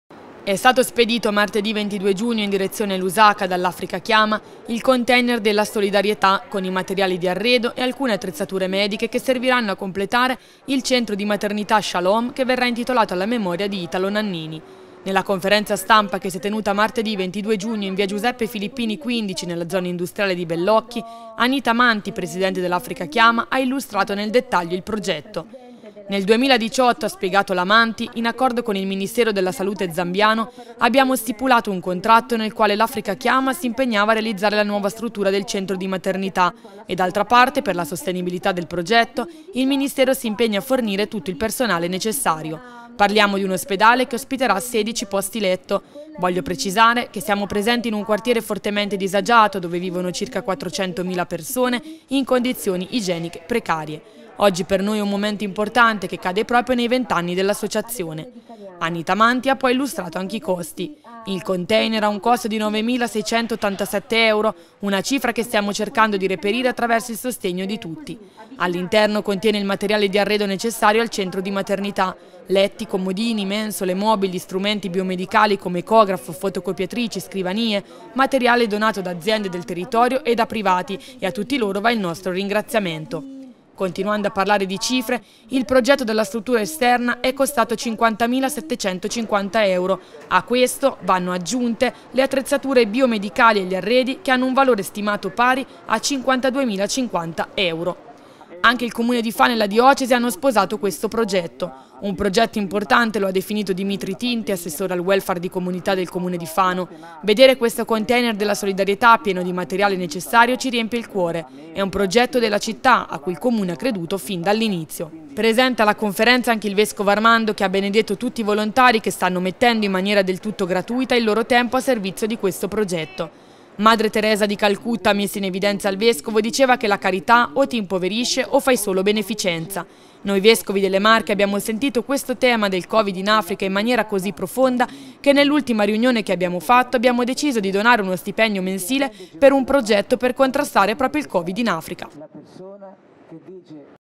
Partito il container della solidarietà
E’ stato spedito martedì 22 giugno in direzione Lusaka dall’Africa Chiama il container della solidarietà con i materiali di arredo e alcune attrezzature mediche che serviranno a completare  il Centro di Maternità Shalom che verrà intitolato alla memoria di Italo Nannini.